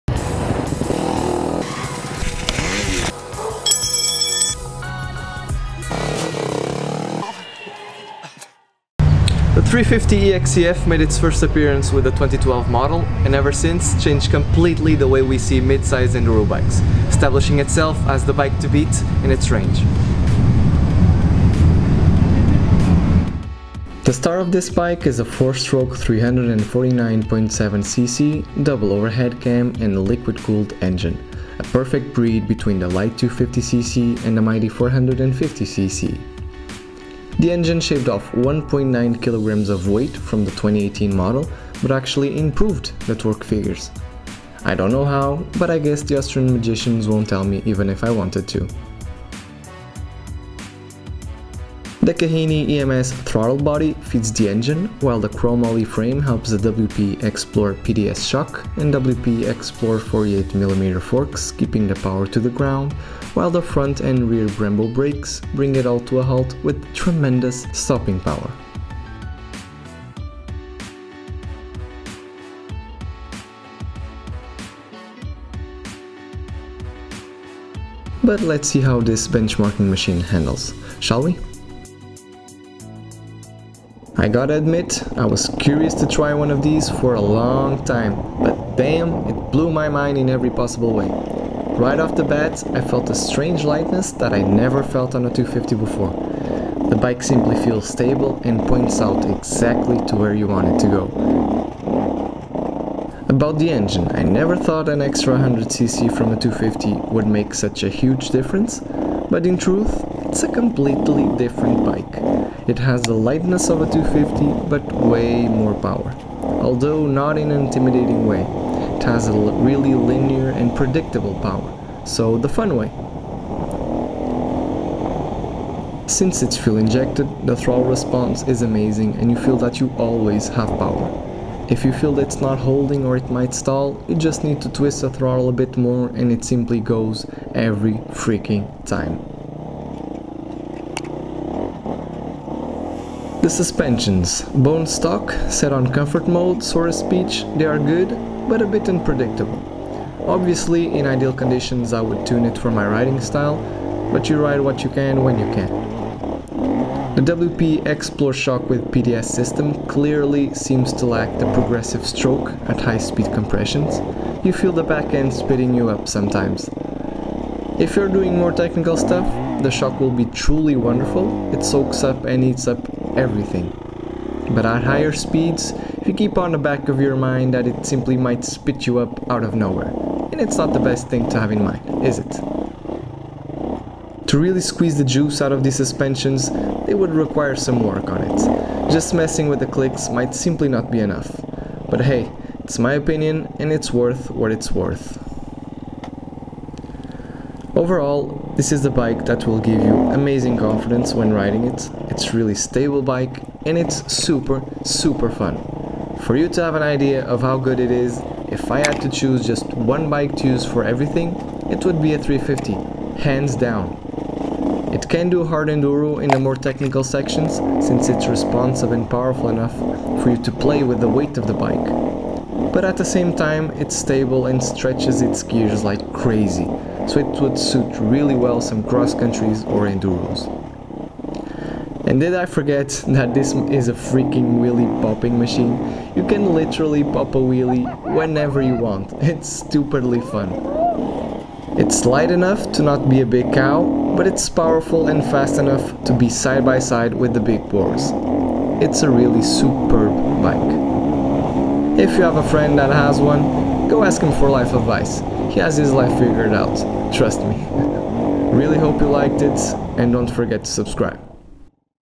Review: KTM 350 EXC-F Six Days 2019
KTM 350 EXC-F completely changed the way we see a mid-sized engine enduro bike, establishing itself as the bike to beat within its range. Thanks to Freeride Spirit, I had the chance to try the 2019 model and give it a good spin.

Here's my honest review about the KTM 350 EXC-F (spoiler alert: I loved the freaking bike!).

Follow the S2P channels to keep updated on all the videos and news!

Cheers!